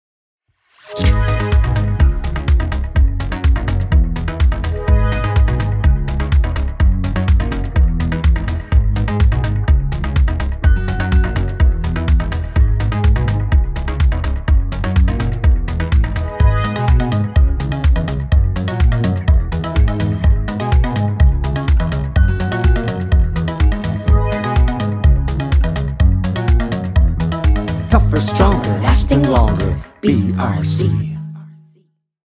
Is Your Roof Ready for Hurricane or Storm-Force Winds?
We carried out a test to see how strong a BRC roof is and if it could provide suitable protection in the event of hurricane or storm-force winds.